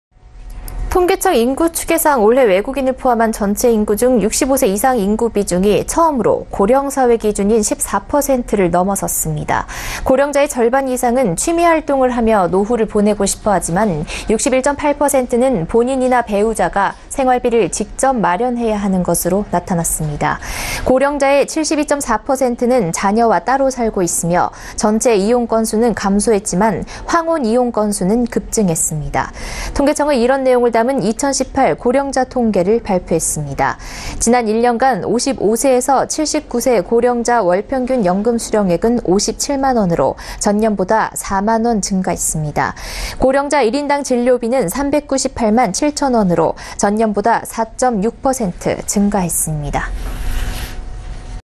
[서울경제TV] 65세이상 62%는 생활비 ‘직접 마련’
65세 이상 인구 최초로 고령사회 기준 넘어서
61.8% ‘본인이나 배우자가 생활비 직접 마련’
72.4% ‘자녀와 따로 살아’… 황혼이혼 급증
월평균 연금 수령 57만원… 1인당 진료비 398만원

통계청 인구 추계 상으로 올해 외국인을 포함한 전체인구 중 65세 이상 인구 비중이 처음으로 고령사회 기준인 14%를 넘어섰습니다.
고령자의 절반 이상은 취미활동을 하며 노후를 보내고 싶어하지만, 61.8%는 본인이나 배우자가 생활비를 직접 마련해야 하는 것으로 나타났습니다.
고령자의 72.4%는 자녀와 따로 살고 있으며, 전체 이혼 건수는 감소했지만, 황혼이혼 건수는 급증한 것으로 나타났습니다.
통계청은 이런 내용을 담은 ‘2018 고령자통계’를 발표했습니다.
지난 1년간 55∼79세 고령자 월평균 연금 수령액은 57만원으로 전년보다 4만원 증가했습니다. 고령자 1인당 진료비는 398만7,000원으로 전년보다 4.6% 증가했습니다.